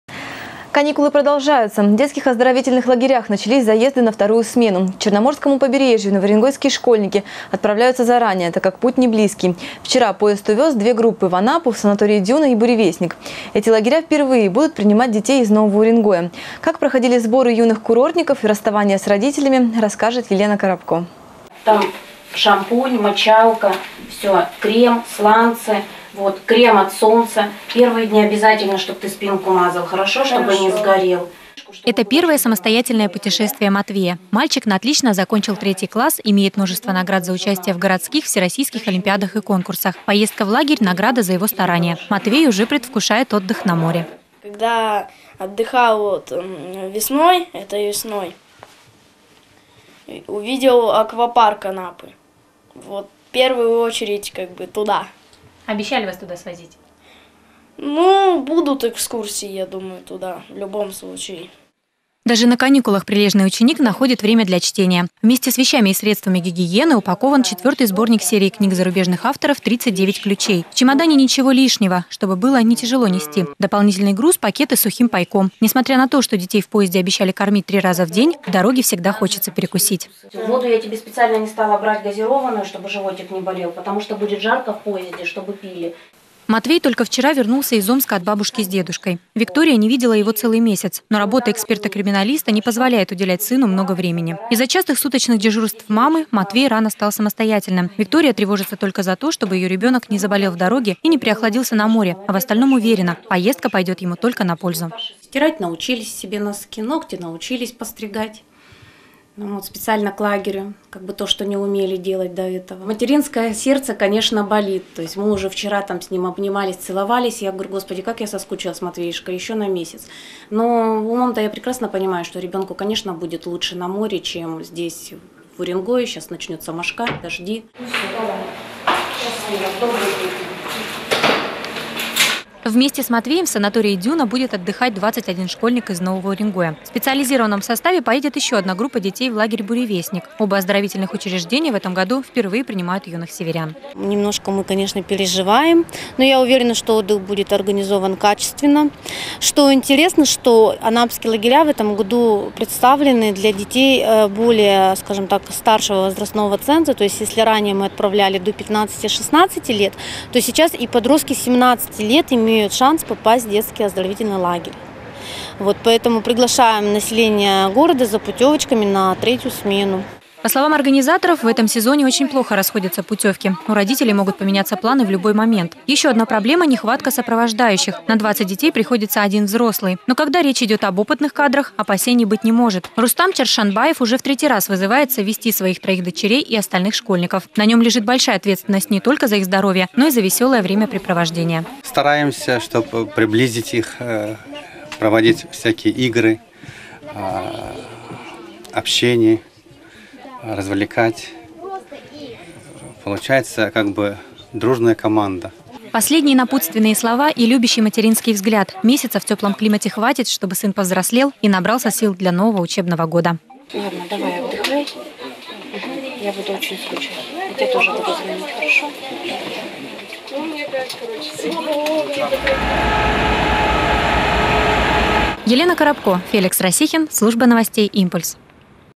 Юные новоуренгойцы отправились в оздоровительные легеря на вторую смену.
Каникулы продолжаются. В детских оздоровительных лагерях начались заезды на вторую смену. К Черноморскому побережью новоуренгойские школьники отправляются заранее, так как путь неблизкий. Вчера поезд увёз две группы в Анапу, в санатории «Дюна» и «Буревестник». Эти лагеря впервые будут принимать детей из Нового Уренгоя.